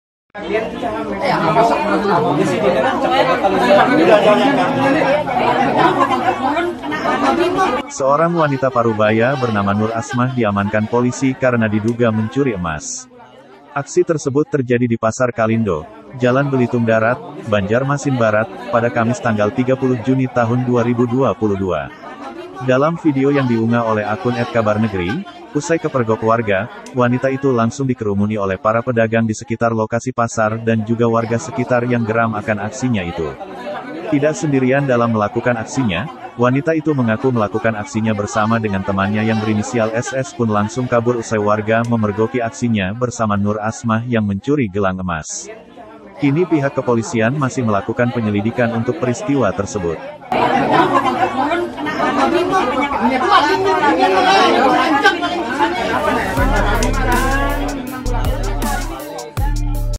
Aksi Emak-emak Nyolong Emas di Banjarmasin
WARTAKOTALIVE.COM, JAKARTA --Seorang wanita paruh baya bernama Nur Asmah (49) diamankan polisi karena  diduga mencuri emas.

Aksi tersebut terjadi di Pasar Kalindo, Jalan Belitung Darat, Banjarmasin Barat, pada Kamis (30/06/2022)

Dalam video yang diunngah oleh akun @kabarnegri, usai kepergok warga, wanita itu langsung dikerumuni oleh para pedagang di sekitar lokasi pasar dan juga warga sekitar yang geram akan aksinya itu.

Tidak sendirian dalam melakukan aksinya, wanita itu mengaku melakukan aksinya bersama dengan temannya yang berinisial S.

Video Jurnalis: Instagram @kabarnegri
Penulis: Joanita Ary
Editor Video: Choirudin Cahyadi